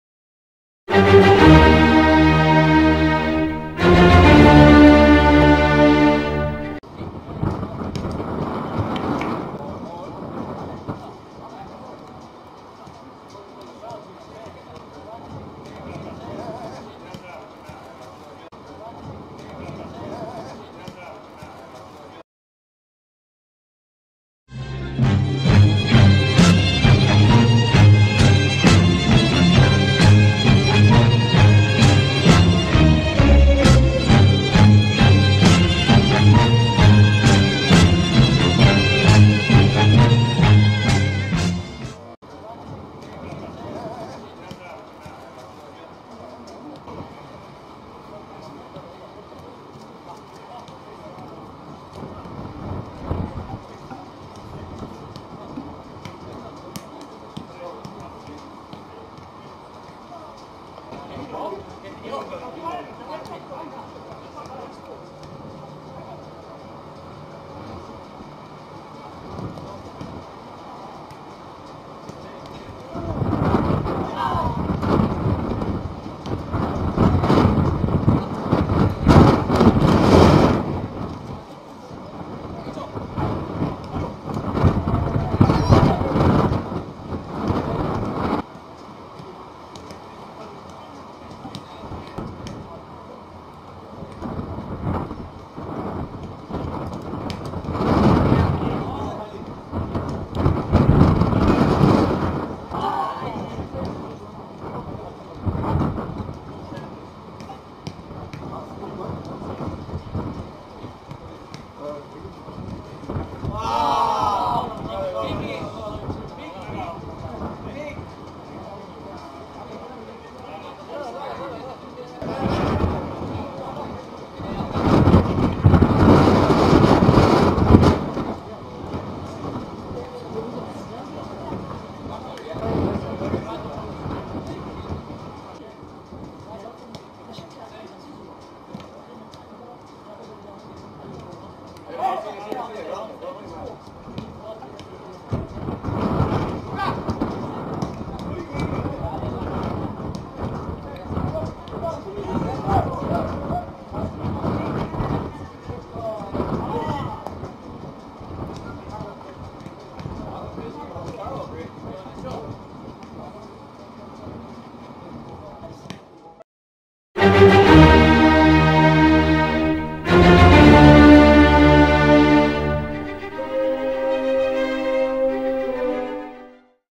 Ketika Ronaldo Dipermalukan Oleh Skill Zidane Saat Latihan
Hightlight pertandingan sepakbola terpopuler